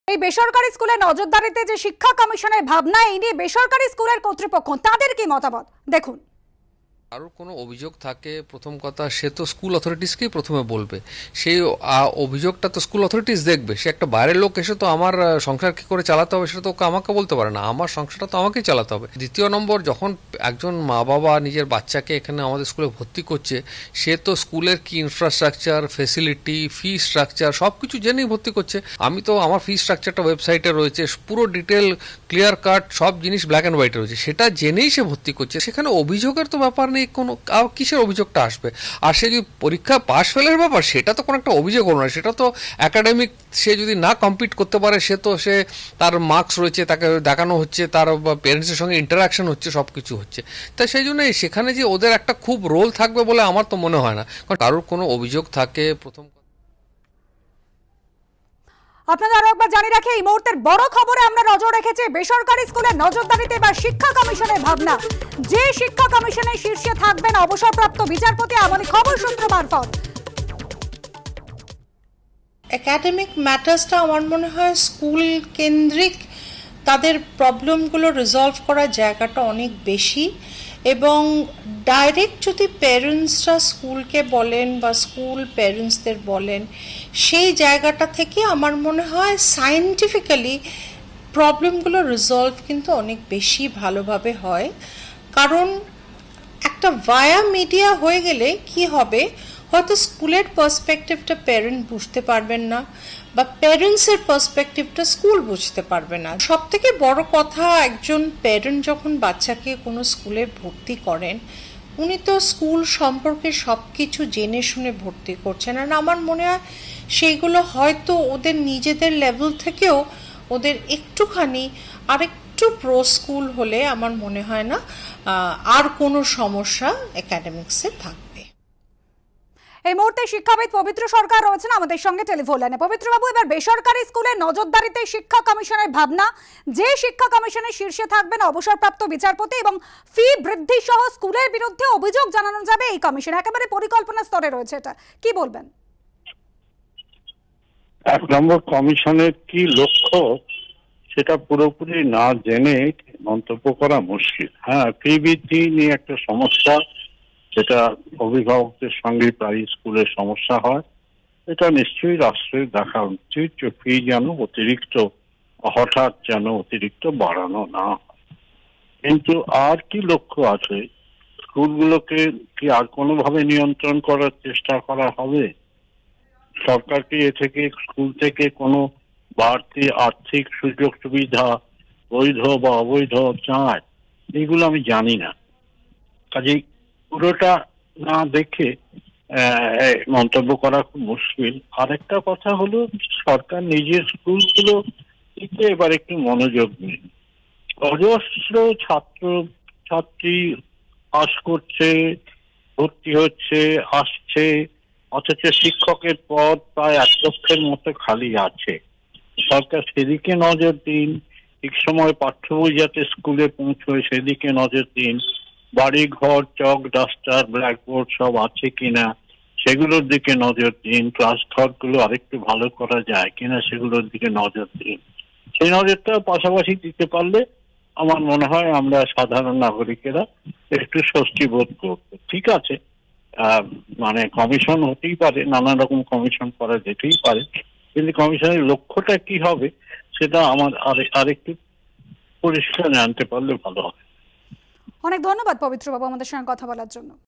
Supriyo Dhar: বেসরকারি স্কুলের নজরদারিতে এবার শিক্ষা কমিশন, কী বলছেন লা মার্টিনিয়ারের সচিব?
বেসরকারি স্কুলের নজরদারিতে এবার শিক্ষা কমিশন? শিক্ষা কমিশনের শীর্ষে থাকবেন অবসরপ্রাপ্ত বিচারপতি, খবর সূত্রের। ফি বৃদ্ধি-সহ স্কুলের বিরুদ্ধে অভিযোগ জানানো যাবে কমিশনে।
স্কুল সংক্রান্ত অভিযোগ খতিয়ে দেখে পদক্ষেপ করবে কমিশন। শিক্ষা কমিশন নিয়ে মিলেছে নবান্নের সবুজ সঙ্কেত, সূত্রের খবর। পরিকল্পনা স্তরে রয়েছে শিক্ষা কমিশন, জানালেন শিক্ষামন্ত্রী।
এই প্রসঙ্গে লা মার্টিনিয়ারের সচিব সুপ্রিয় ধর জানান, "কোনও অভিযোগ থাকলে তা স্কুল কতৃপক্ষ দেখবে। বাইরের লোক এসে আমাকে বলতে পারে না, আমার সংসার কী করে চালাতে হবে। এখানে শিক্ষা কমিশনের কিছু করণীয় থাকবে বলে আমার মনে হয় না।"